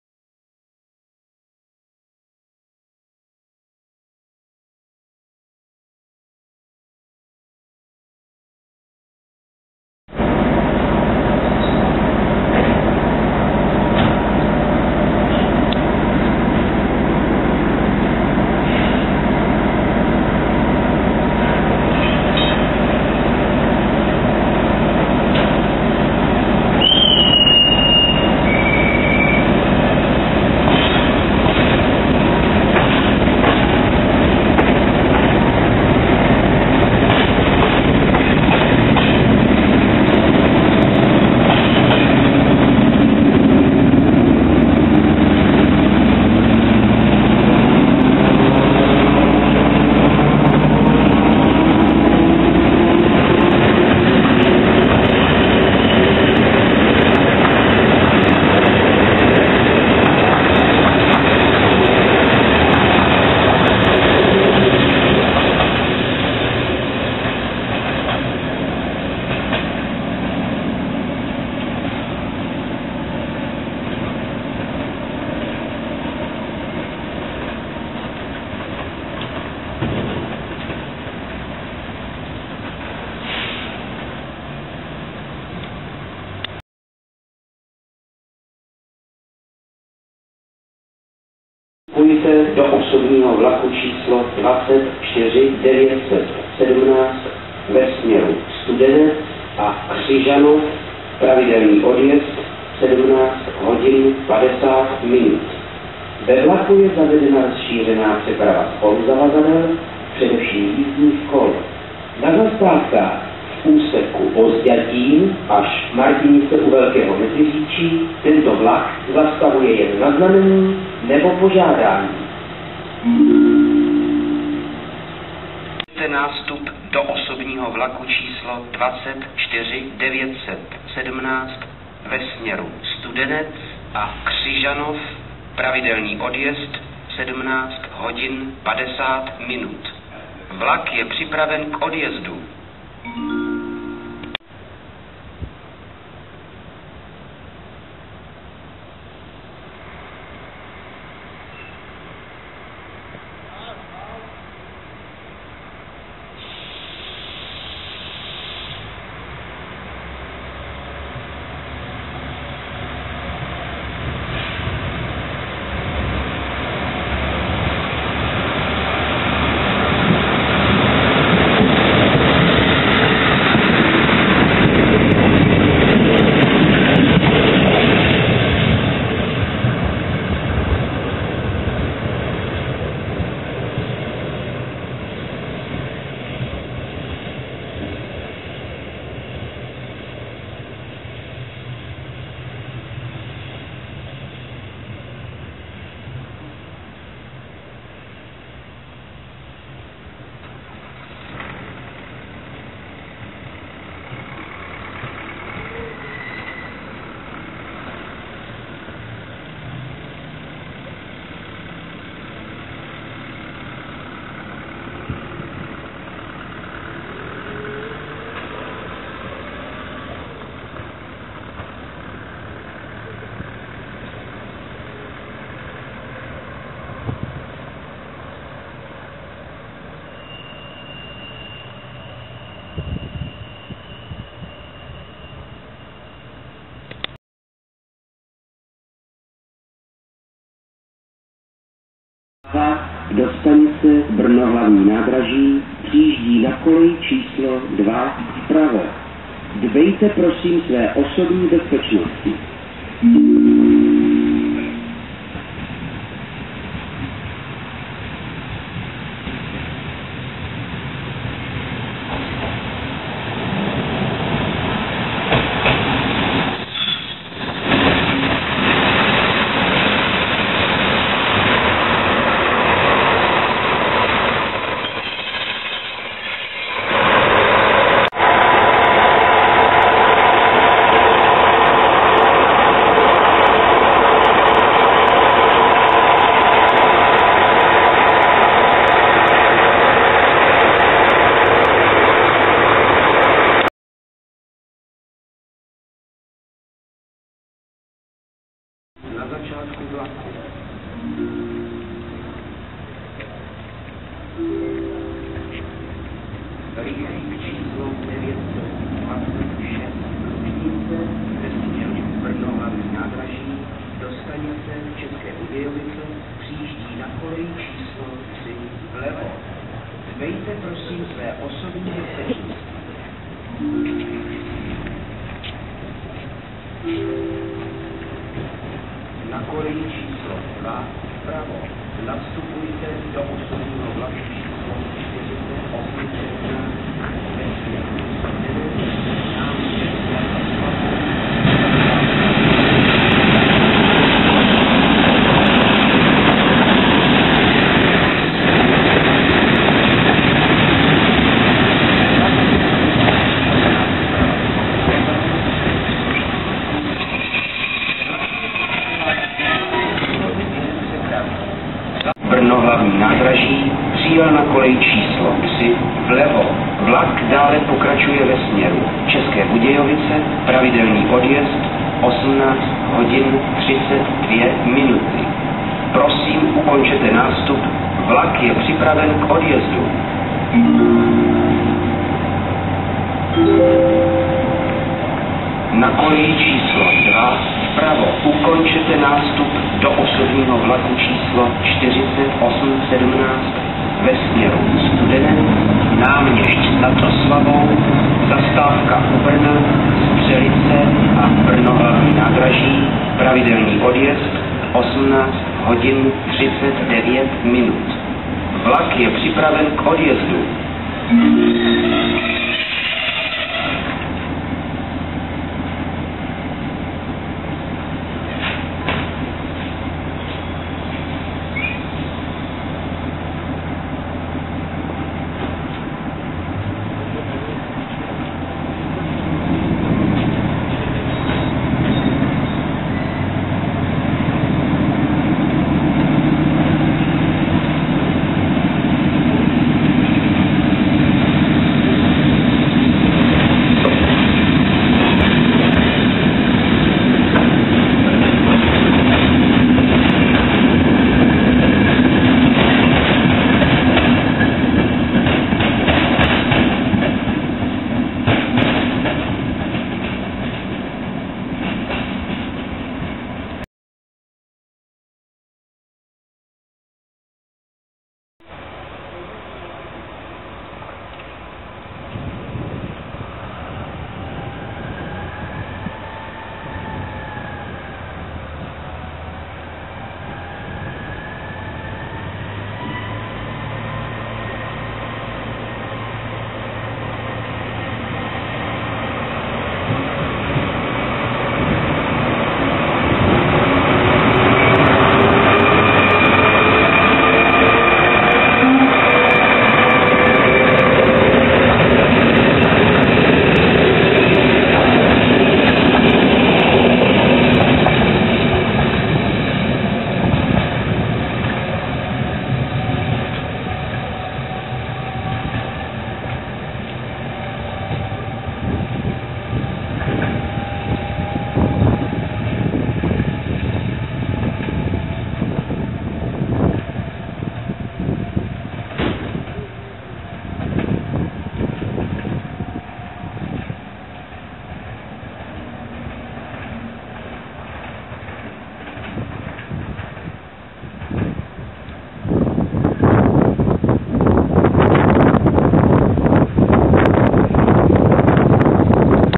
Železniční stanice Třebíč ve tmě, 29. 1. 2009
Železniční stanice Třebíč dne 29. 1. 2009, tentokráte za temného večera. Popis videí:
1. R 663 "Vajgar" (Plzeň - Brno) a Os 4816 (Brno - Jihlava), odjezdy vlaků ze stanice. V čele obou vlaků lokomotiva řady 754 ("brejlovec").
2. RegioNova 814.011 na Os 24917 ve směru Studenec, Velké Meziříčí a Křižanov.
3. Os 4817 (Jihlava - Brno), příjezd vlaku do stanice. Hnacím vozidlem je "hydra" 854.025.
4. R 926 "Lužnice" (Brno - České Budějovice) - příjezd a odjezd vlaku.
5. Os 4817, odjezd vlaku ve směru Brno hl. n.